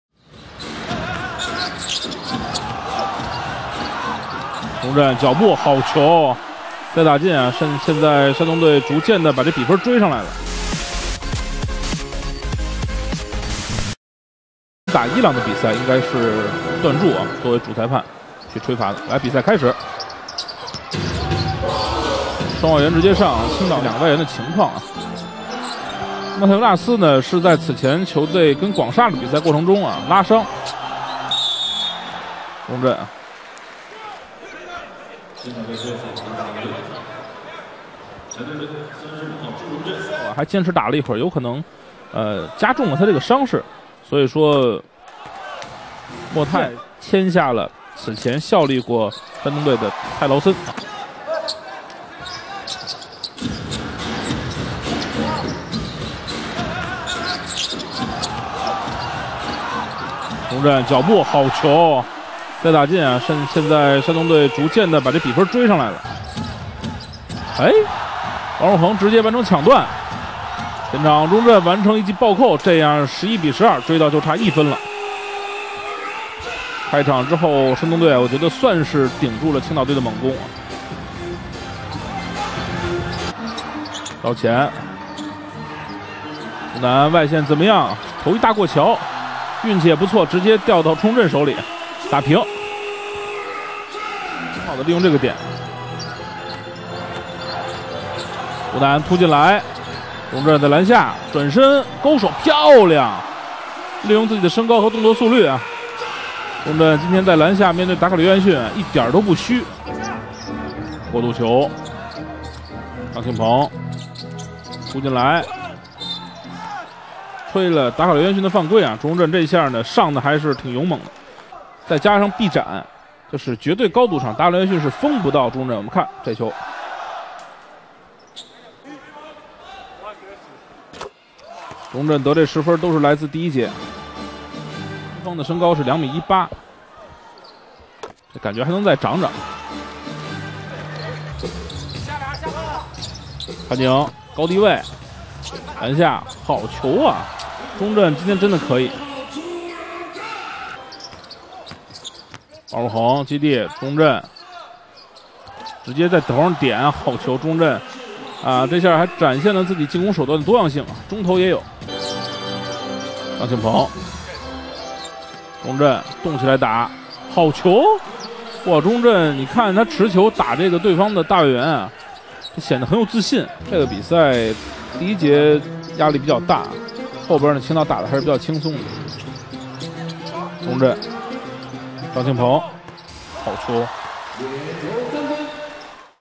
国青最强中锋！山东19岁朱荣振18分9篮板个人集锦 | 生涯新高 | CBA常规赛季
朱荣振，1999年5月19日生于山东省济南市章丘区，中国职业篮球运动员，司职中锋，现效力于山东高速篮球俱乐部。
2017年9月，朱荣振代表山东高速篮球俱乐部出战中国男子篮球职业联赛。
2015年，朱荣振入选国家男子篮球青年队。2017年，朱荣振入选国家男子篮球集训红队。